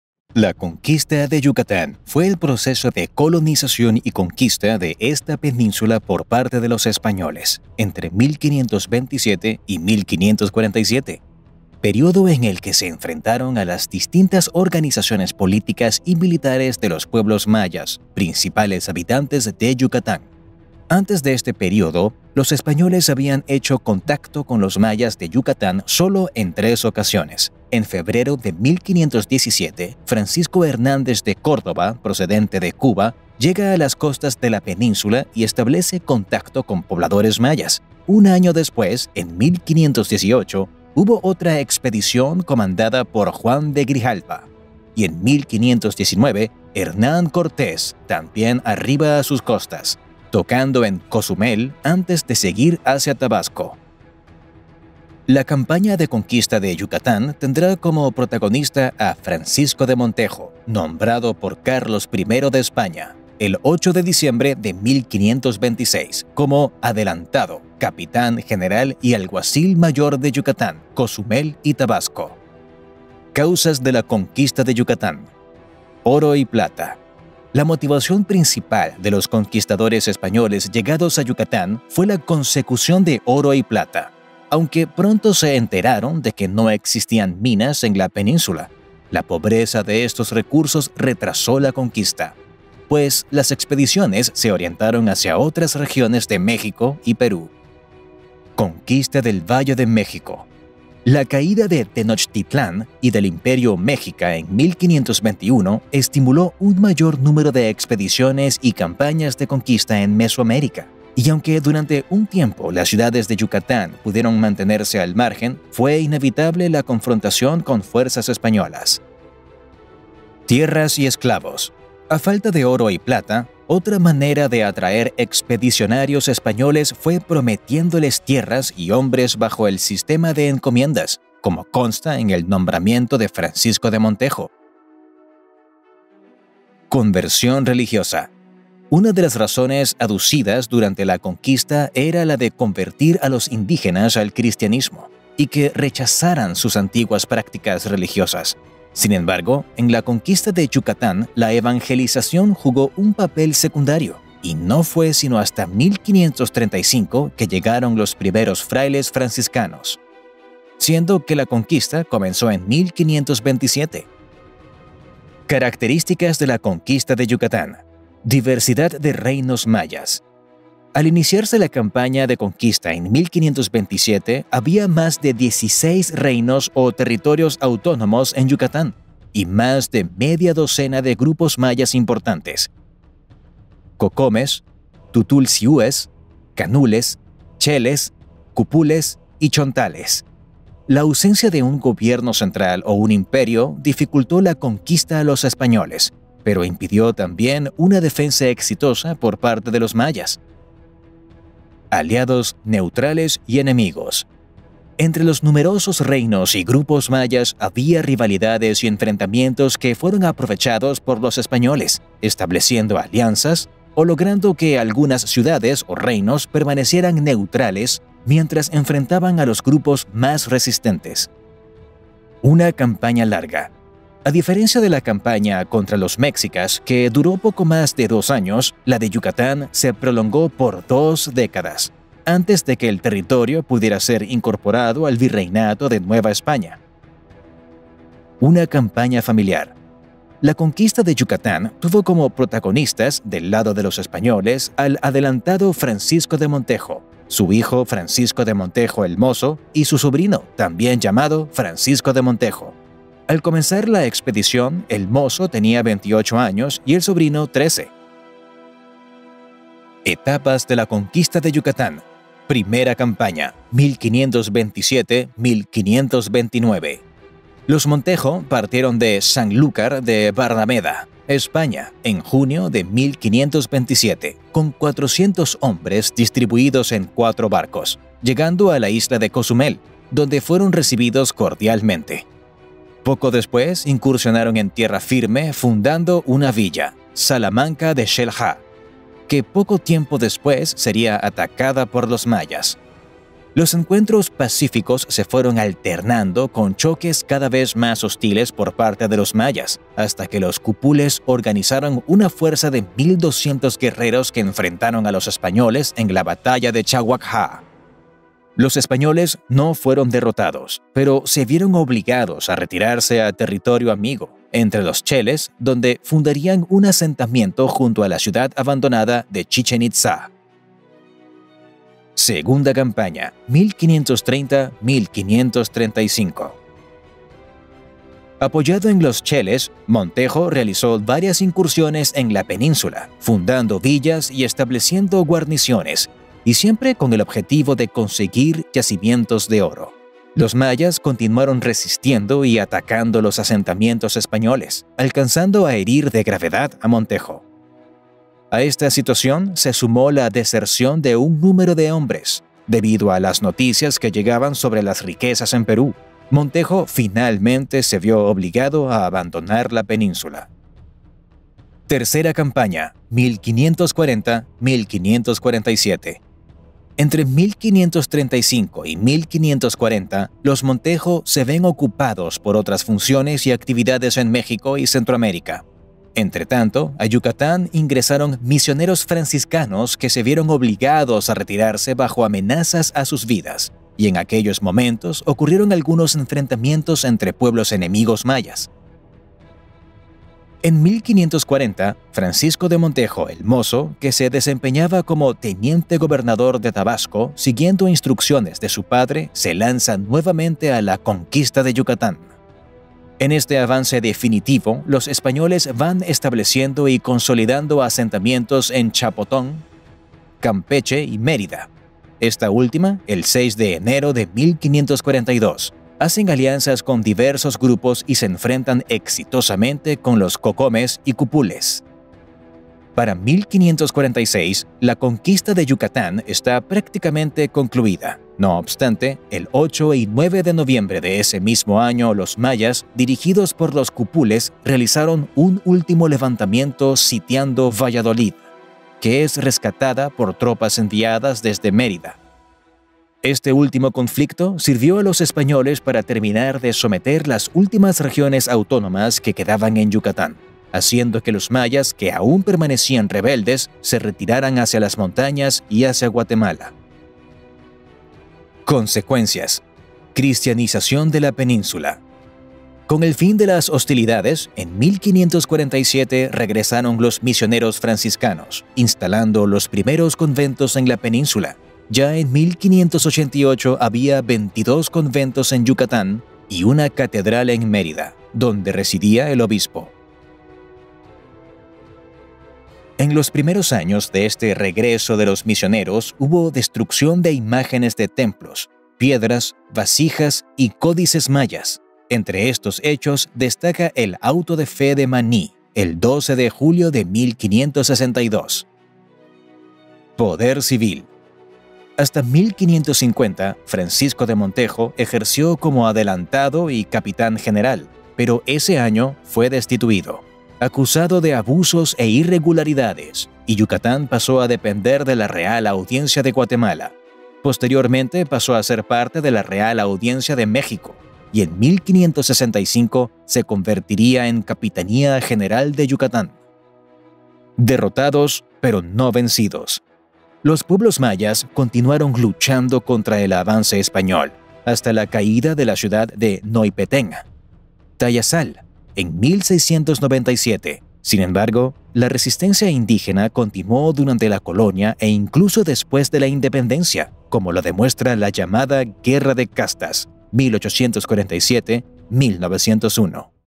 La CONQUISTA DE YUCATÁN: sus causas, etapas y consecuencias⚔️
La conquista de Yucatán; explicamos en qué consiste, sus características, causas, etapas, consecuencias y personajes principales.